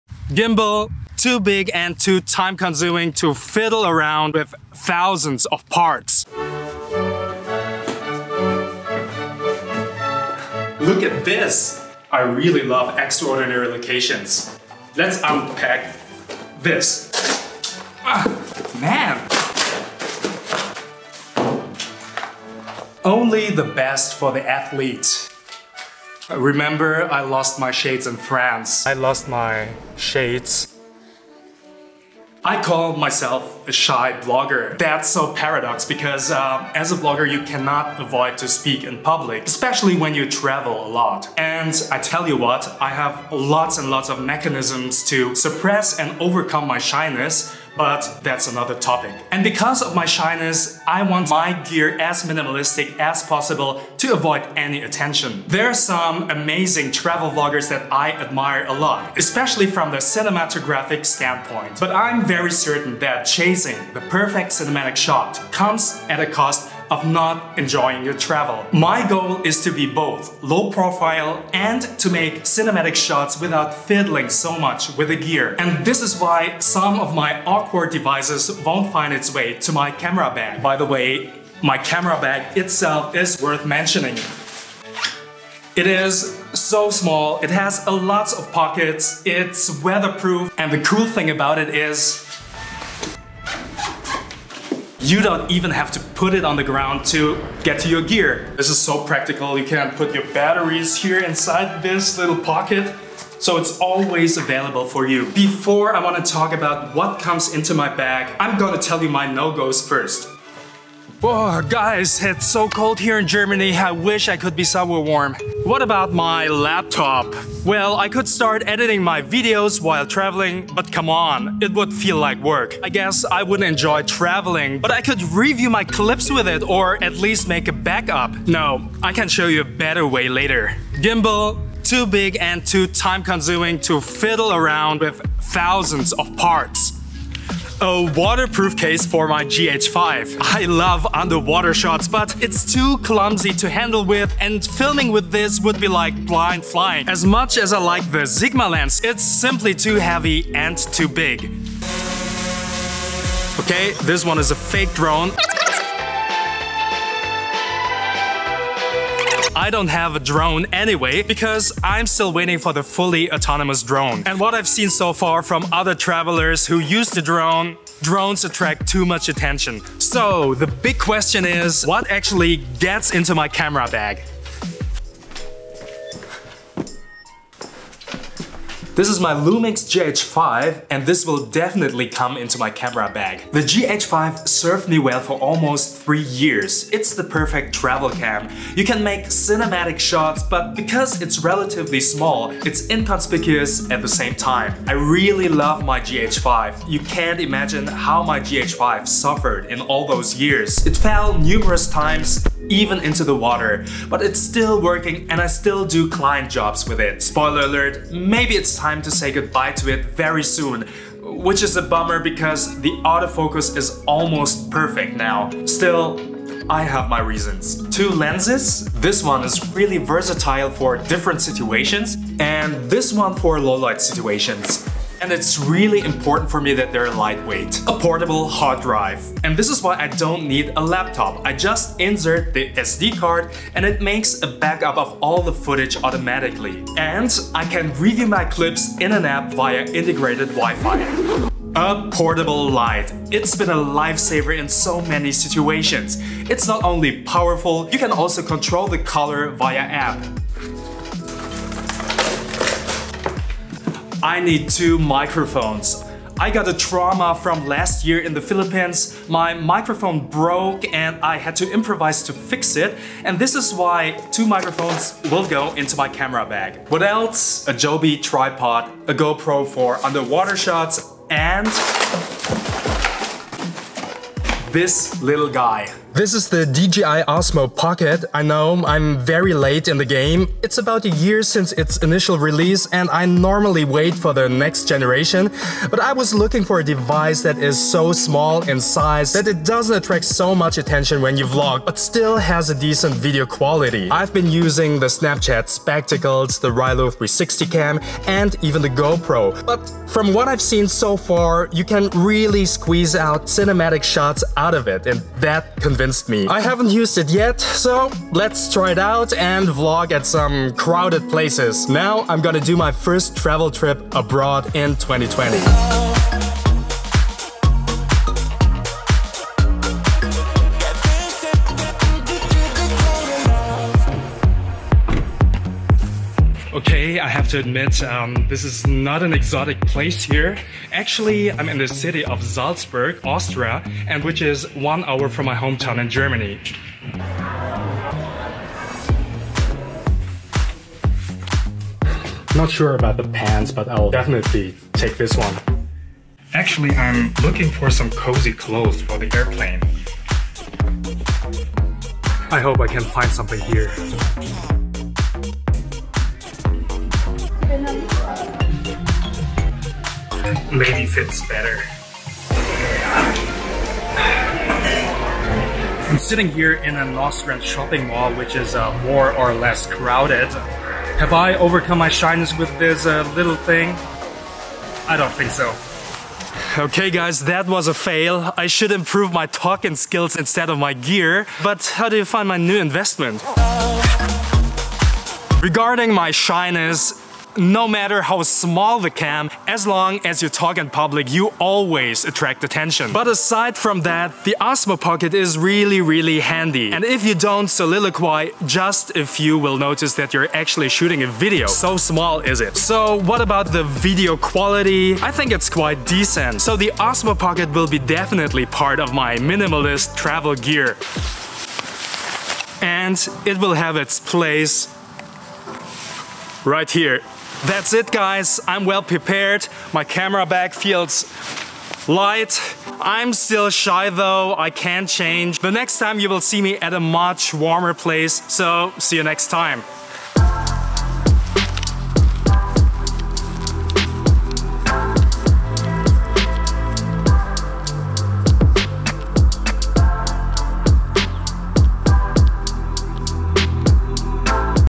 Minimalist CAMERA GEAR 2020 For Shy Travel Vloggers
Enjoy your travel by keeping your travel video gear as minimalistic as possible. More electronic equipment means more devices to charge and more things to take care of.

And for those of you who are shy to talk into your camera in public like me small devices don’t attract too much attention. At least in theory.

What do you think. Which of these possible devices will find it’s way into my camera backpack (Lowepro Flipside Trek BP 350 AW):

∙ Røde VideoMicro
∙ Røde VideoMic GO
∙ DJI Ronin-SC
∙ Panasonic Leica DG Vario-Elmarit 12-60mm
∙ Panasonic Leica DG Summilux 12mm
∙ Sigma 18-35mm
∙ WD My Passport Wireless SSD
∙ Aputure MC
∙ SeaFrogs Underwater Housing

Now guess what’s my ultimate camera kit 😎